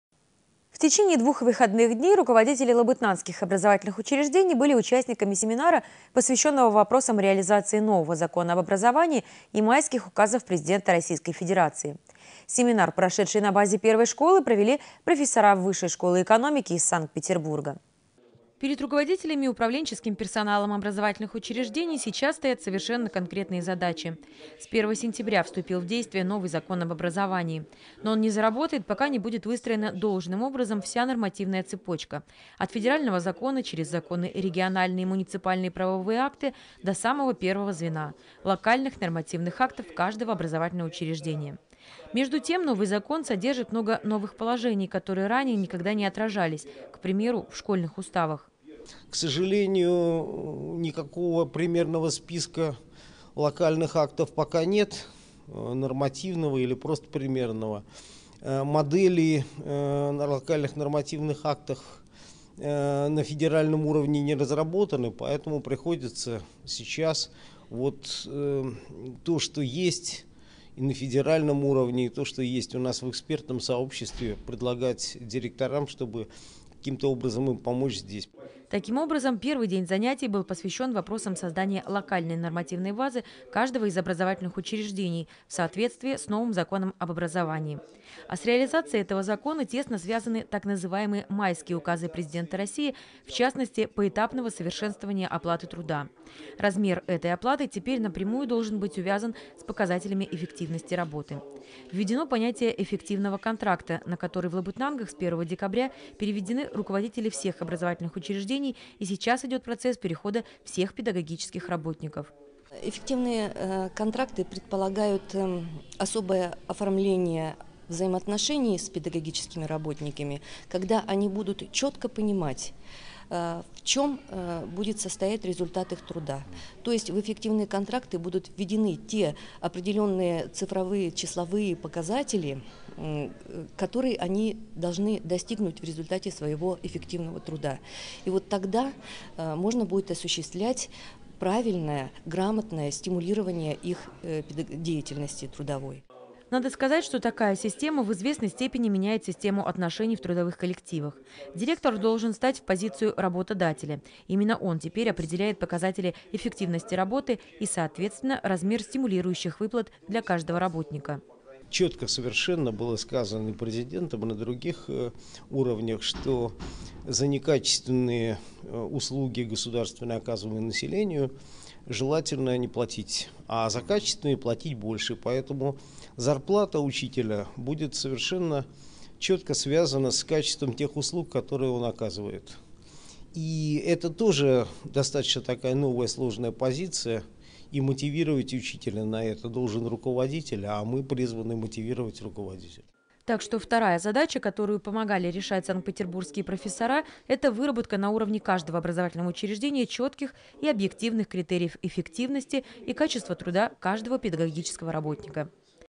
От общего к частному
Профессора Высшей школы экономики из Санкт-Петербурга провели для лабытнангских педагогов семинар, посвященный новому Закону об образовании.